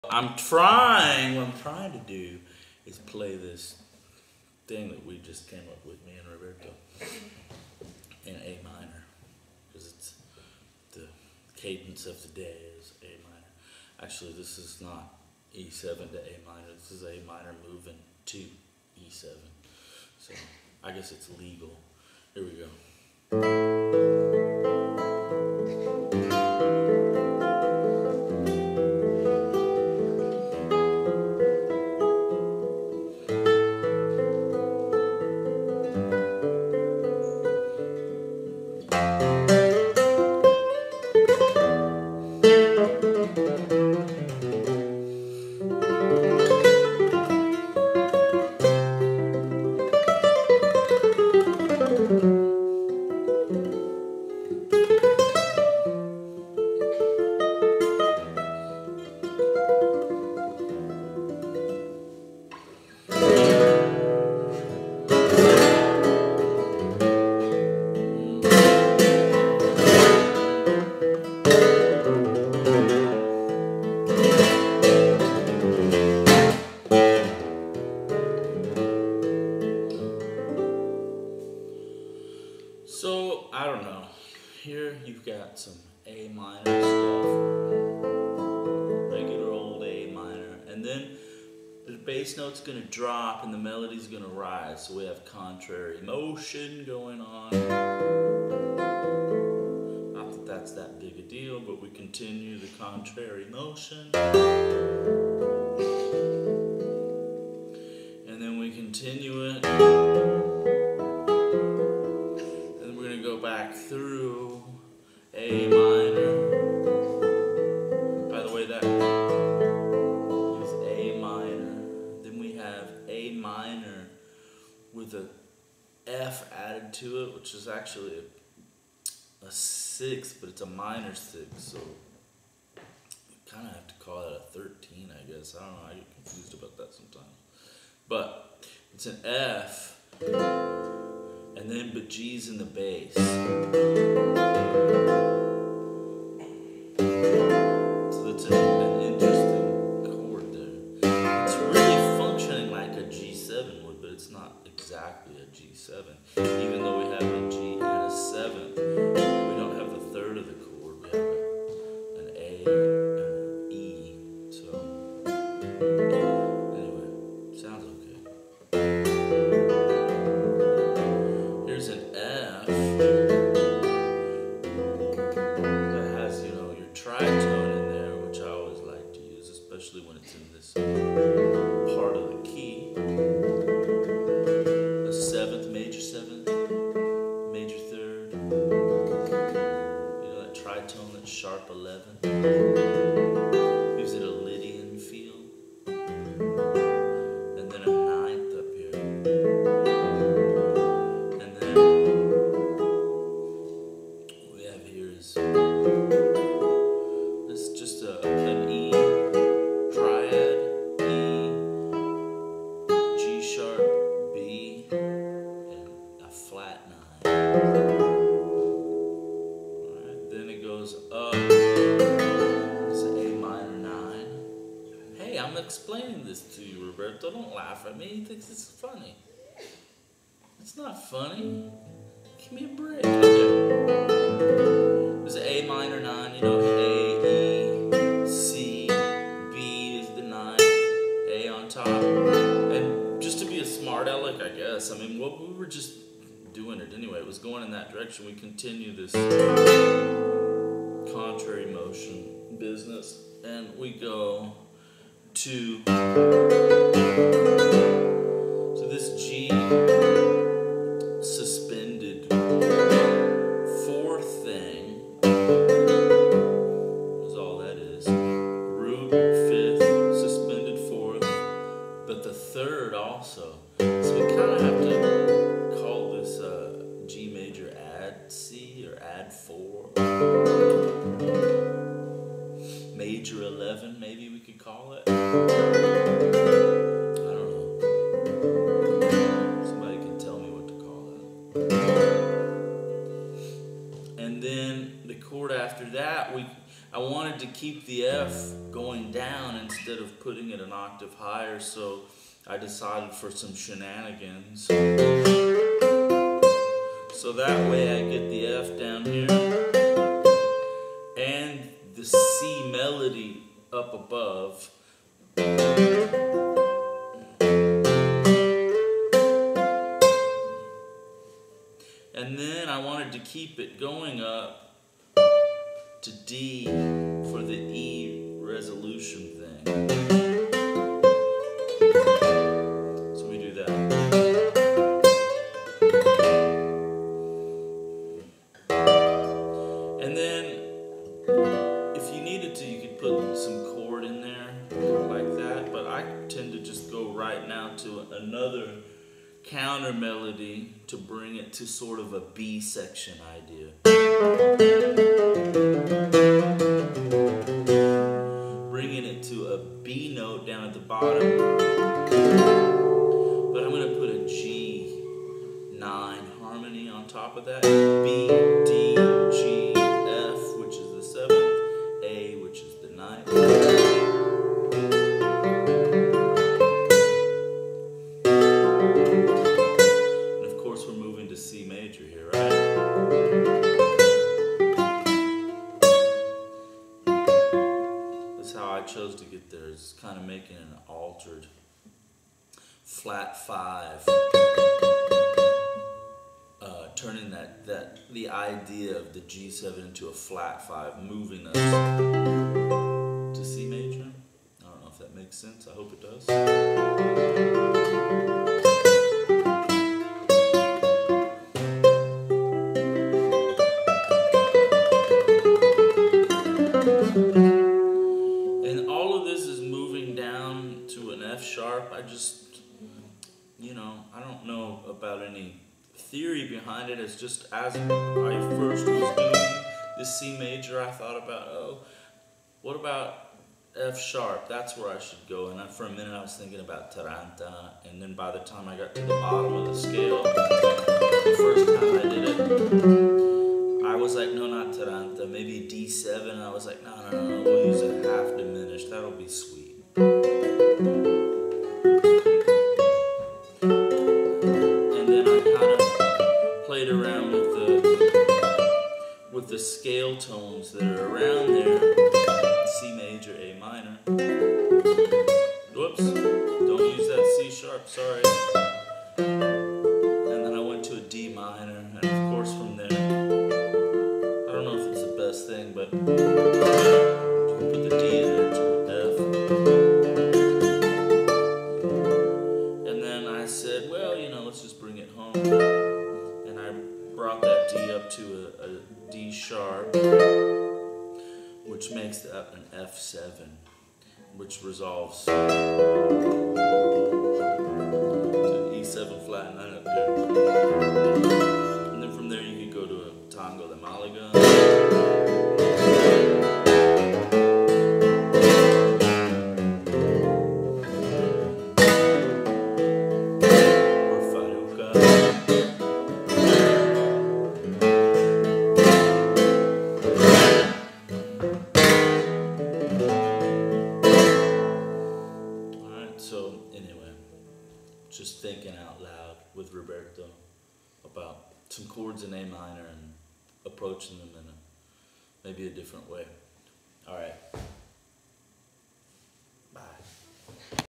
Another Private Lesson With Roberto Part 1
A minor harmony ideas, possible Farruca or Tangos de Malaga